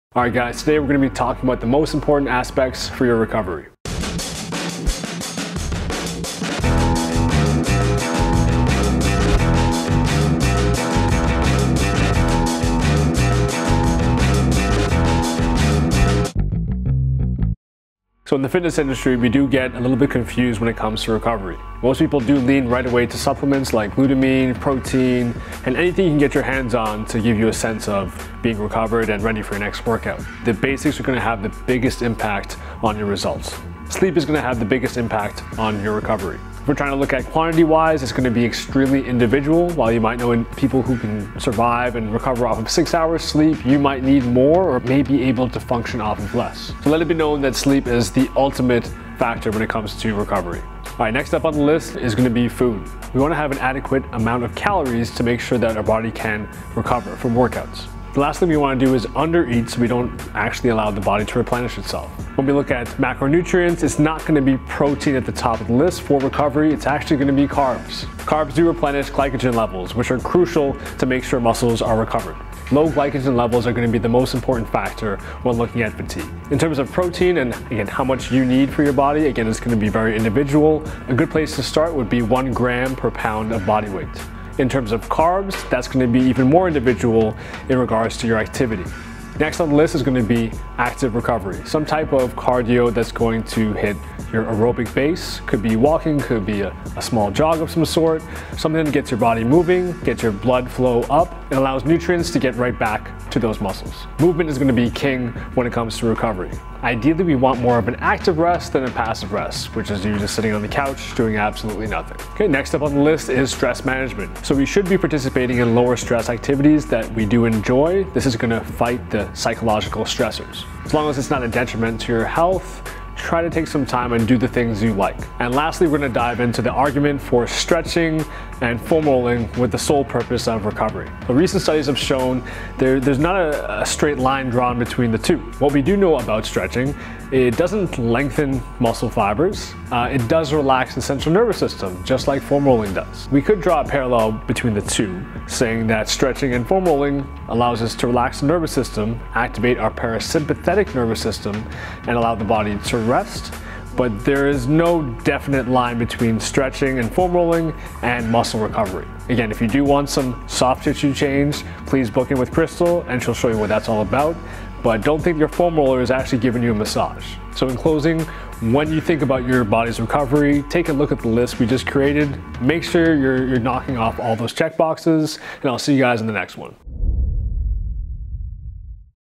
Staples: Recovery
Your 𝗴𝗮𝗶𝗻𝘀 from training will only be as good as your 𝗿𝗲𝗰𝗼𝘃𝗲𝗿𝘆.  No matter how intense your training is; your recovery must 𝗺𝗮𝘁𝗰𝗵 it.

..And  if we’re all after 𝗮𝗻𝗮𝗯𝗼𝗹𝗶𝘀𝗺, who’s better to use an example than a 𝗴𝗿𝗼𝘄𝗶𝗻𝗴 puppy?

1) 𝗦𝗹𝗲𝗲𝗽: the most natural and effective
- lowers cortisol, increases testosterone (this is a good thing)

2) 𝗙𝗼𝗼𝗱: what fuels you and your recovery
- the lower your calories dip under maintenance, the harder it is to recover

3) 𝗔𝗰𝘁𝗶𝘃𝗶𝘁𝘆: doesn’t have to be a step master session
- some light / aerobic cardio will benefit your body, don’t make an excuse

4) 𝗦𝘁𝗿𝗲𝘀𝘀 𝗠𝗴𝘁: if lifting is your only hobby, it’d help to have another
- while most of us lift to relieve stress, it pays to have a life outside the gym, with what you enjoy

5) 𝗦𝘁𝗿𝗲𝘁𝗰𝗵𝗶𝗻𝗴 / 𝗙𝗼𝗮𝗺 𝗥𝗼𝗹𝗹𝗶𝗻𝗴? Instead, do yourself a favour and book :@sybingco.rmt
- While they can help to relax the CNS.. these shouldn’t be a focus.